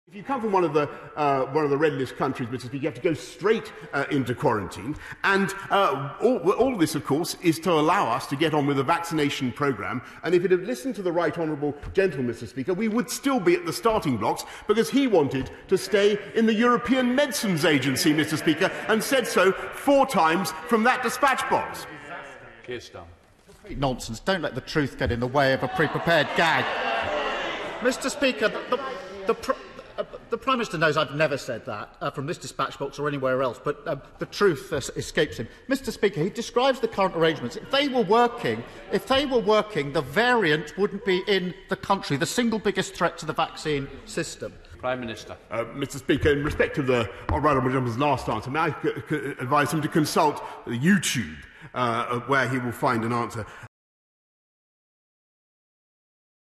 Boris Johnson and Keir Starmer in heated clash over remarks on European Medicines Agency
Sir Keir Starmer has apologised after "mishearing" Boris Johnson and accusing him of speaking "complete nonsense" during a heated exchange in the Commons on Wednesday.

During Prime Minister's Questions, Mr Johnson said Sir Keir had previously supported remaining in the European Medicines Agency (EMA) after Brexit.

The Labour leader furiously denied ever making the comment and accused the Prime Minister of speaking "complete nonsense" for a "pre-prepared gag". But Hansard, the official parliamentary record, shows that in January 2017 he did question why the UK would want to leave the body.

A spokesman later admitted Sir Keir had "misheard" Mr Johnson and believed he had said that Labour supported joining the EU's vaccination programme – a different organisation.